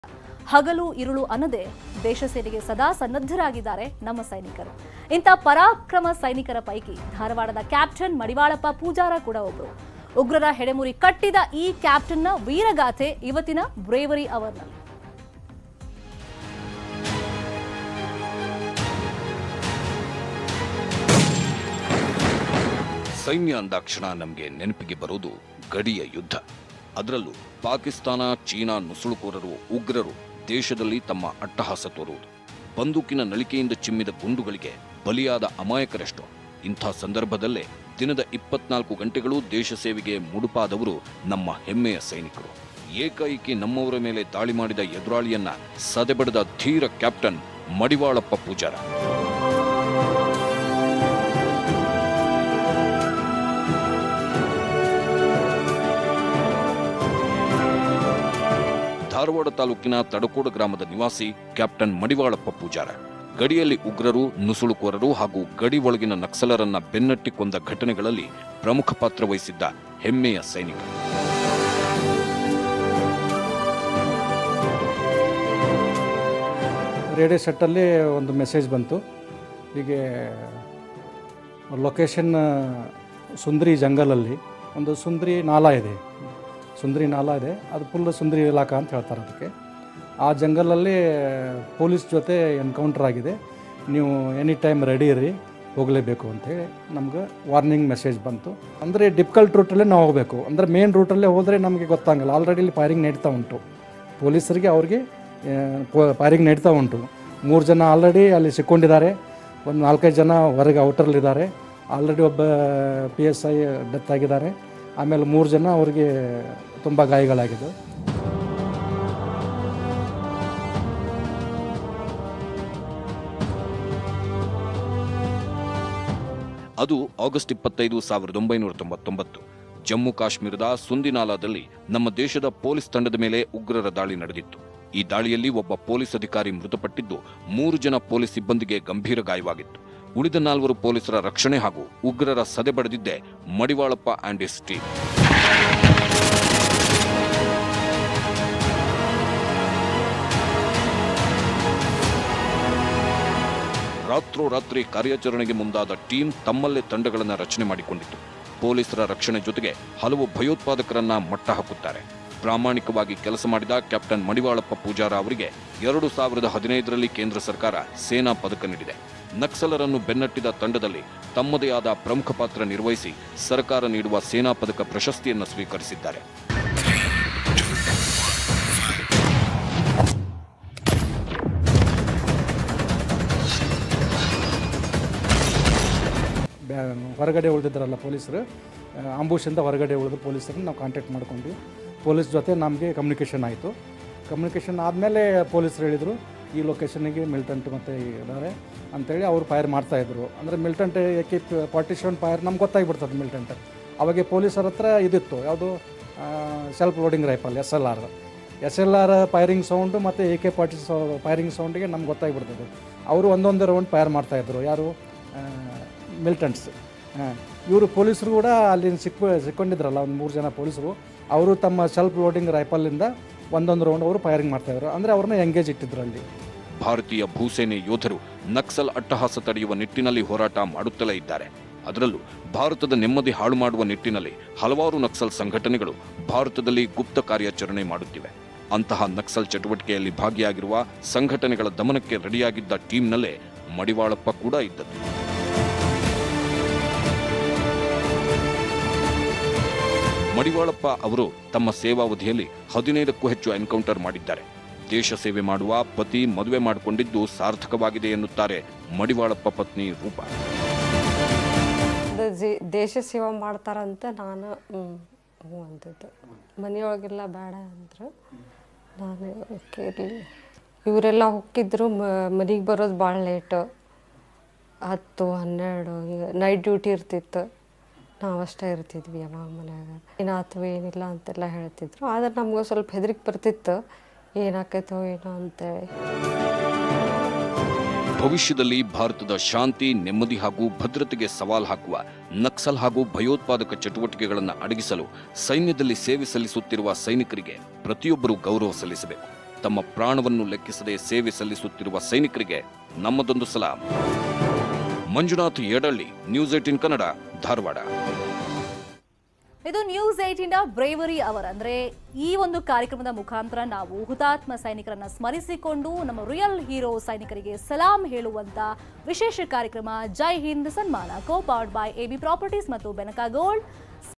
ಜೈ ಹಿಂದ್ ಸನ್ಮಾನ: ನಕ್ಸಲರನ್ನ ಮಟ್ಟಹಾಕಿದ Dharwadನ ಹೆಮ್ಮೆಯ ಸೈನಿಕ ಮಡಿವಾಳಪ್ಪ ಪೂಜಾರರವರ ಯಶೋಗಾಥೆ!
ಜೈ ಹಿಂದ್ ಸನ್ಮಾನ: ದೇಶಕ್ಕಾಗಿ ಸೇವೆ ಸಲ್ಲಿಸಿದ ಹಾಗೂ ನಕ್ಸಲರನ್ನ ಮಟ್ಟಹಾಕಿದ ಧಾರವಾಡದ ಸೈನಿಕ ಮಡಿವಾಳಪ್ಪ ಪೂಜಾರರವರ ಯಶೋಗಾಥೆ ಹೇಗಿದೆ ಗೋತ್ತಾ?